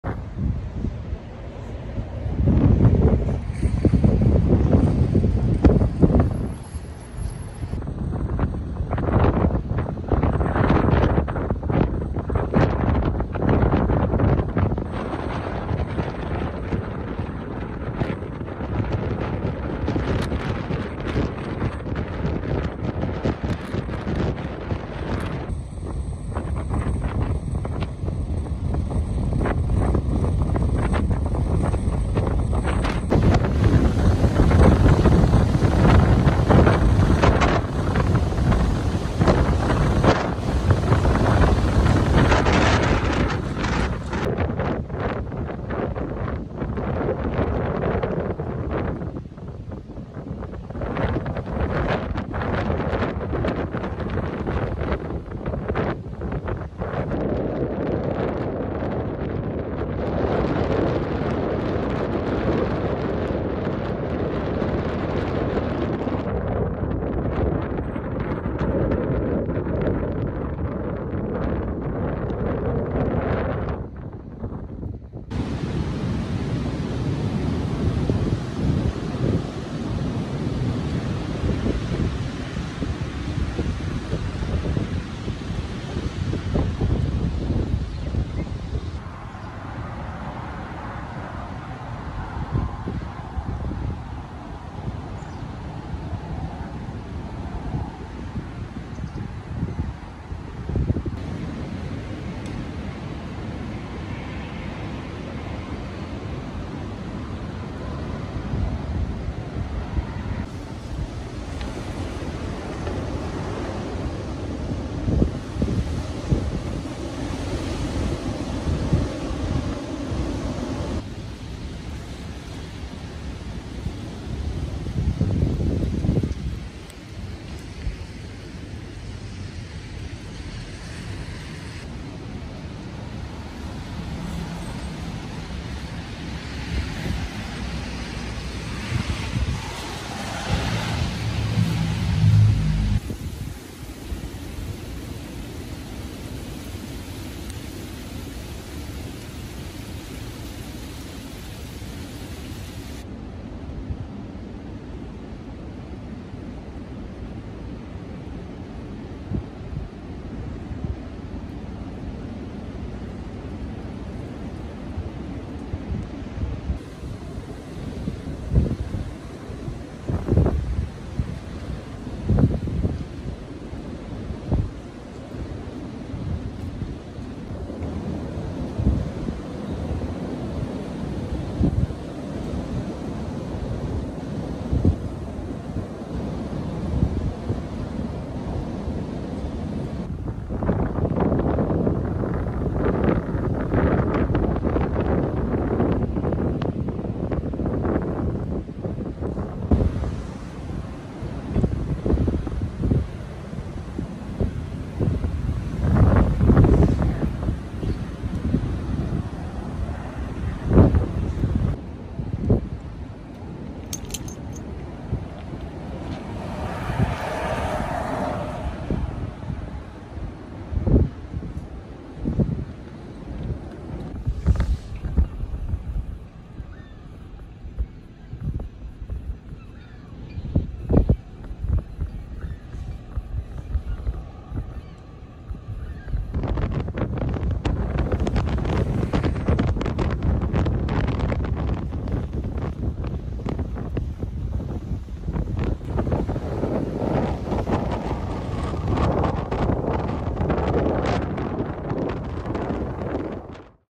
Vermont Shifts From Record Warm To Cold Amid Strong Winds
Record warmth in Vermont came to an end on February 28-29 at the hands of a robust cold front. As seen in this video the front featured strong, sometimes damaging winds ahead of, during and after its passage. Jackson the Weather Dog makes a cameo, but he was not happy with cold post-front winds.